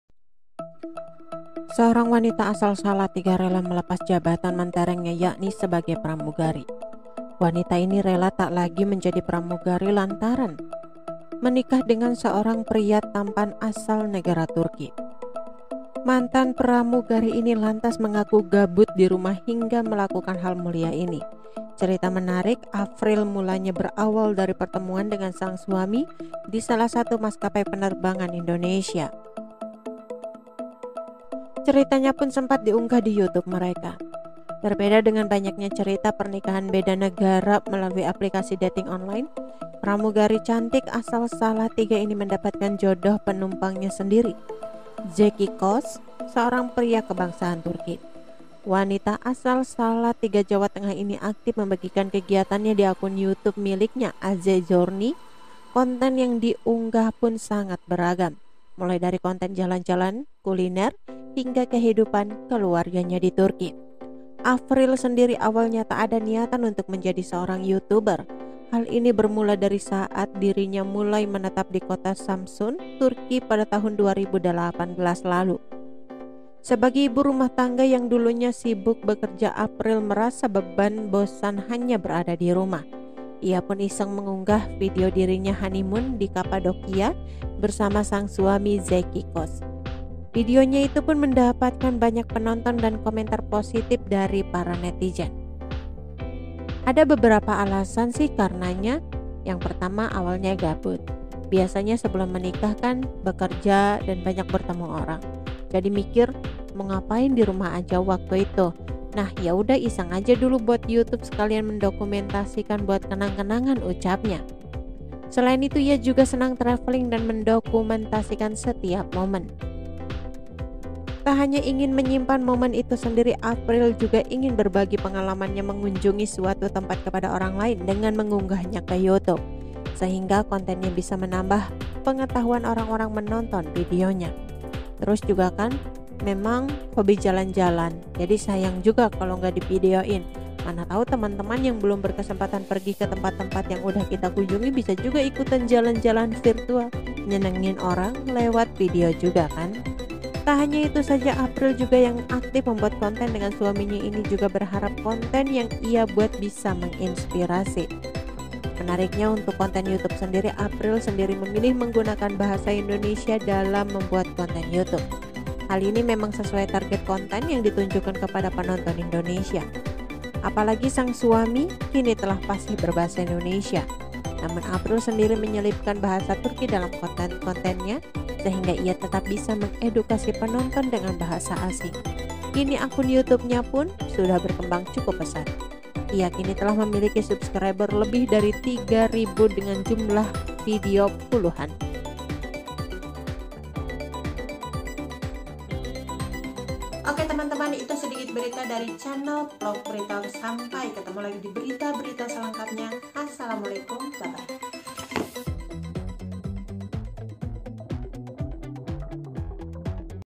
Dinikahi Bule Turki, Eks Pramugari Ini Gabut di Rumah hingga Lakukan Hal Ini untuk Tujuan Mulia
Seorang wanita asal Salatiga rela melepas jabatan menterengnya yakni sebagai pramugari.

Wanita itu rela tak lagi jadi pramugari lantaran menikah dengan seorang pria tampan asal Negara Turki.

Mantan pramugari itu lantas mengaku gabut di rumah hingga lakukan hal mulia ini.